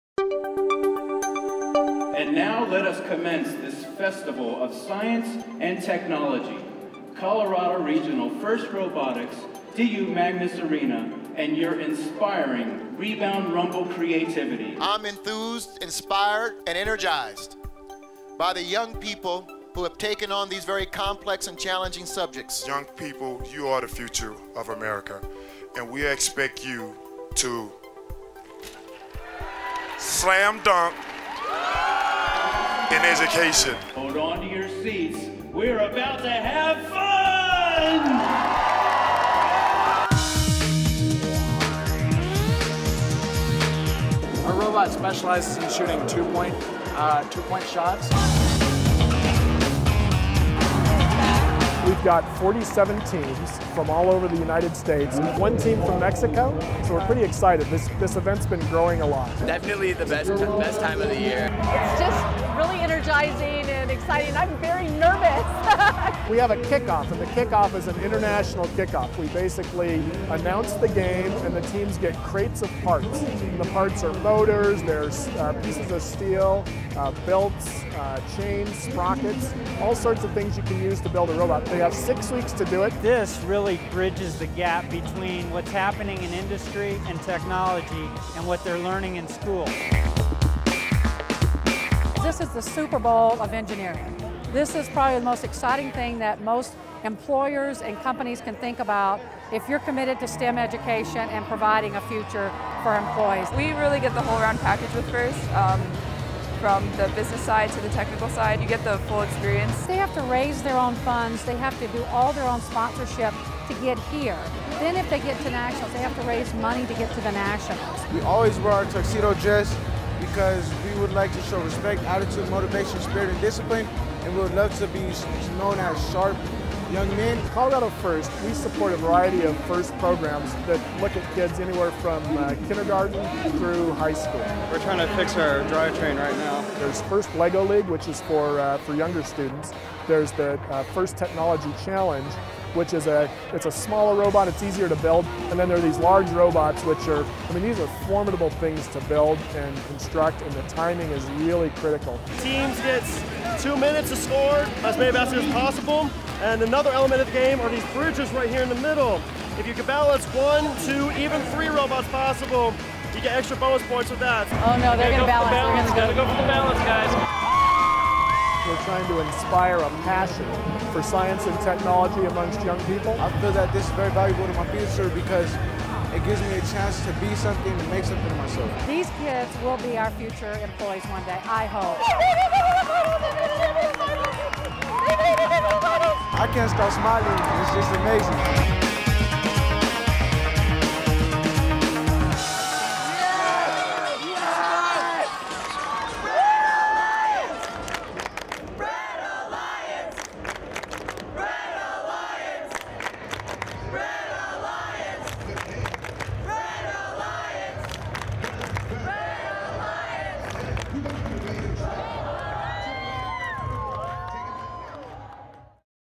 Colorado FIRST Robotics Competition 2012
The Colorado FIRST Regional Robotics Competition challenged teams of talented young people from throughout the United States and Mexico to show off their science and technology skills.  Check out highlights from the Rebound Rumble at the University of Denver's Magness Arena.